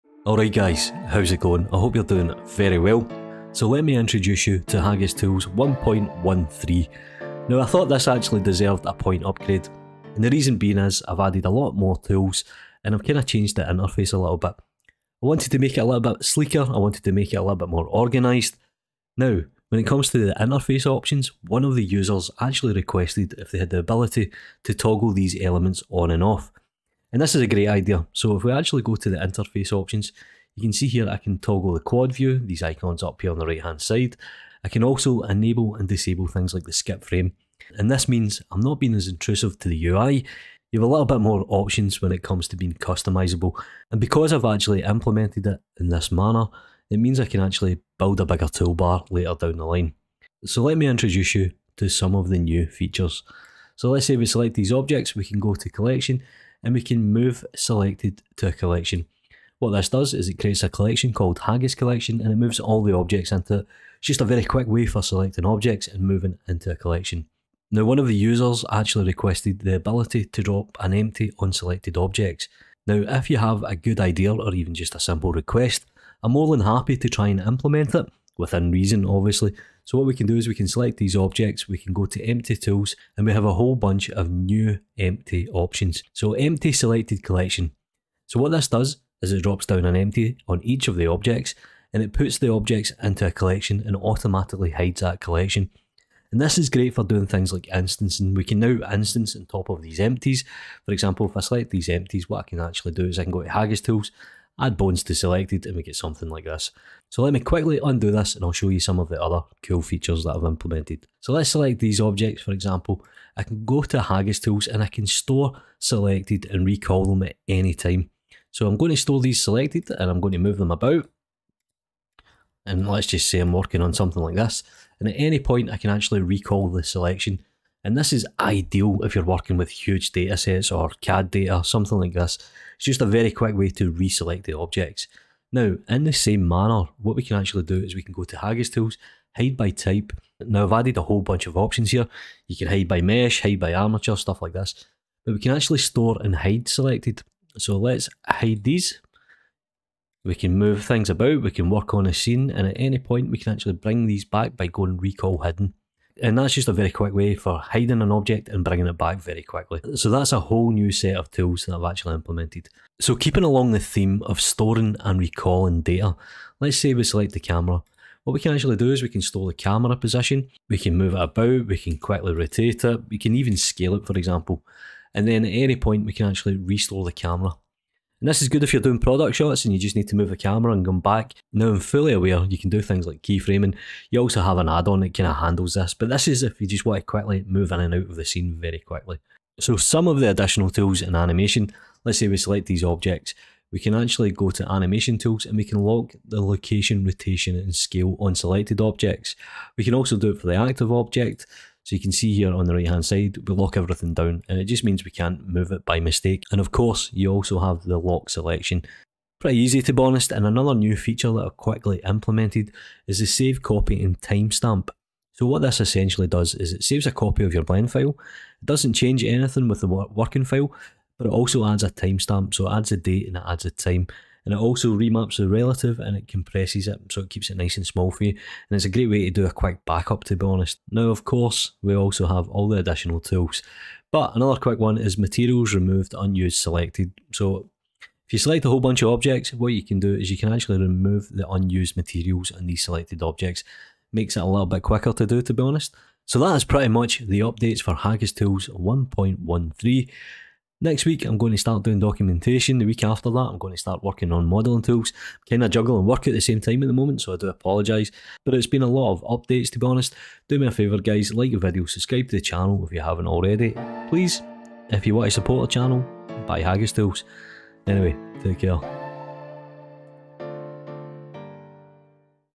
Haggis Tools 1.1.3 Update for Blender Add-on
Buy Haggis Tools on BlenderMarket or Gumroad

Haggis Tools is a Blender add-on 2.8+ geared towards optimizing workflows, reducing mundane and repetitive tasks, and ultimately saving the artists time.
1.1.3 Updated Features
Store and Recall Selected objects Location | Rotation and Scale
Better Ui Control - toggle elements on and off
Quick Move selected objects to collection
Add Empty to selected - perfect for instancing
Store and Hide selected
Hide by Type
Copy and Paste location, rotation, and scale.
Duplicate objects and automatically join.
Open user-specified editor type as a floating window.
Auto smooth shading and enable normal smoothing
Add Bones on selected objects.
Create Empty on selected vertices.
System Tools.
Command-Line Shortcuts.
Match Viewport visibility.
Skip Keyframes by.
Perspective - Camera View and lock camera viewport.
Copy Materials and Unlink.
Quick UI toggles - display name, gizmos.
Create Empty and Parent Selected
Name collection as object name and move
Create Vertex / Uv map with Object name
Rename mesh  object
Toggle QuadView
Ability to customize the toolbar
Open User Specified Editor Type Floating Window... perfect for dual-screen.
Toggle and Display Name of Selected Objects

+ MORE
Lock the Camera View
Toggle Transform, Rotate, and Scale Gizmos